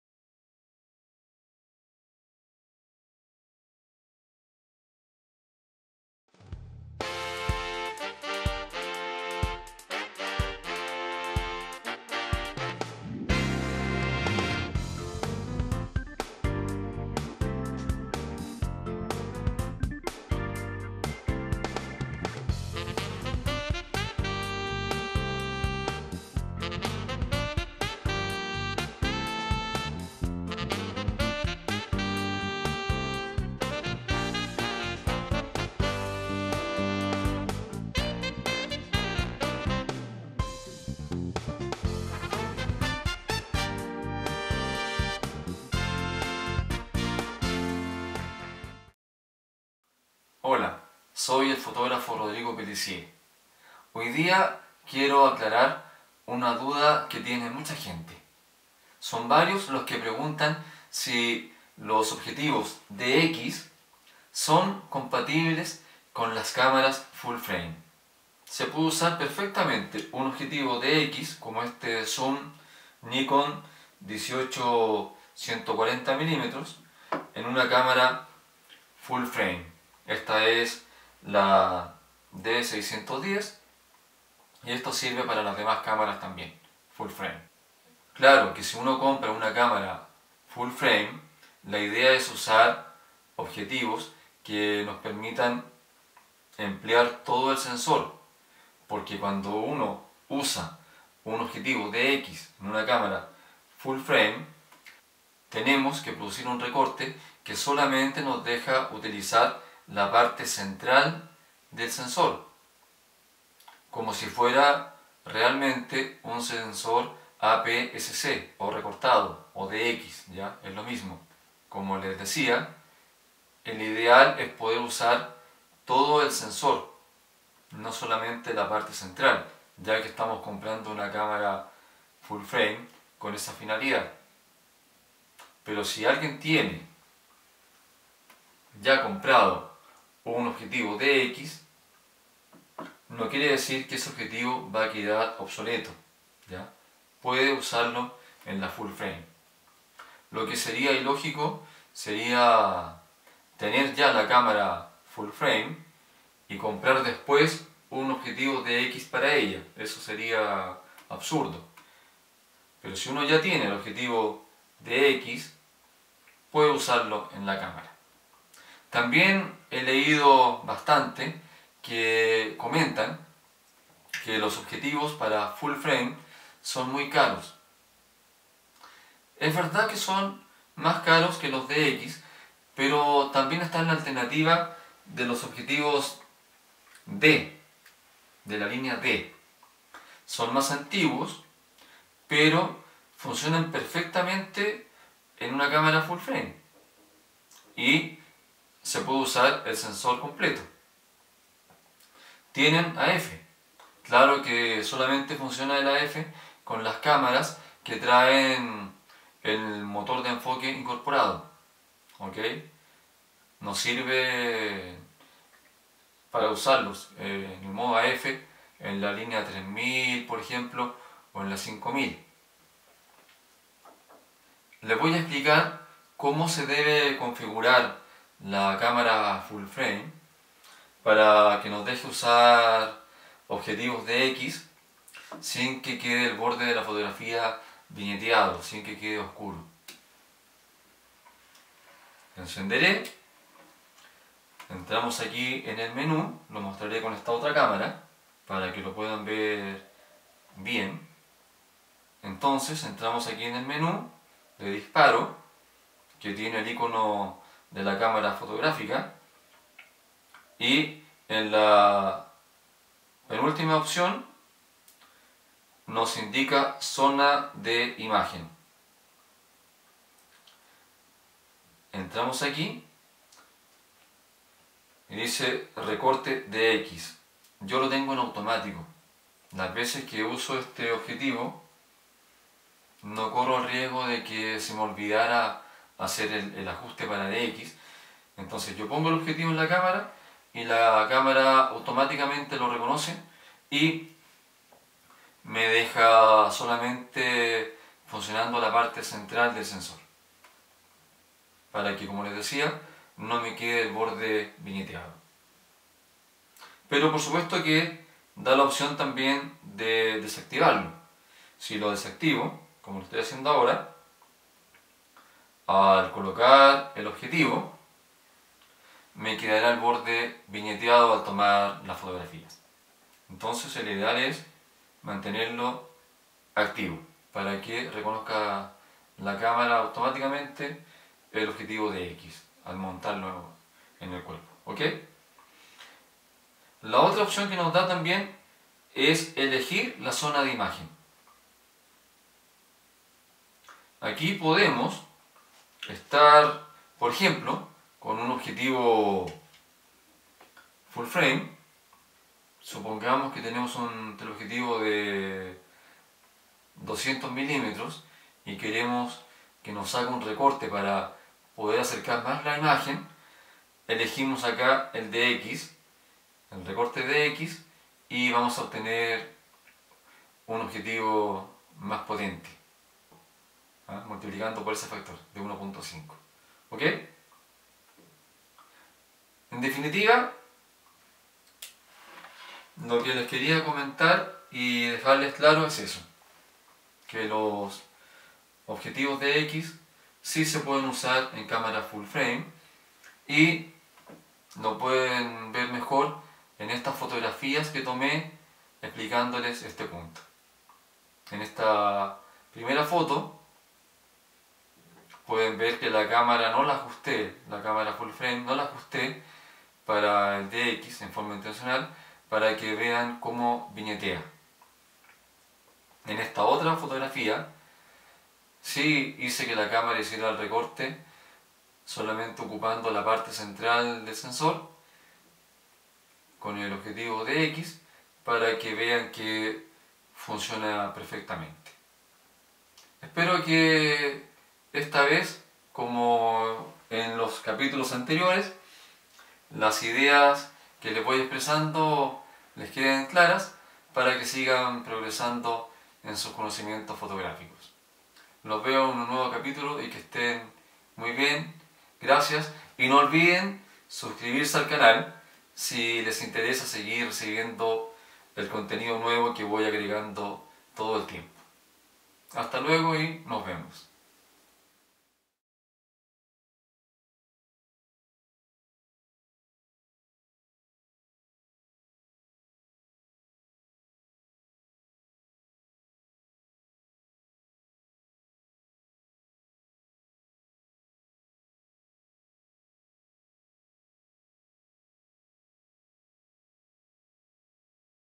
Objetivos para sensor recortado en cámara full frame Tutorial de fotografía 45
TUTORIAL OBJETIVOS PARA SENSORES RECORTADOS En este capítulo del curso de fotografía comento que sí es factible utilizar objetivos DX en cámaras full frame indicando los pasos a seguir para evitar el posible viñeteado en las fotografías Para clases presenciales contáctame en el WhatsApp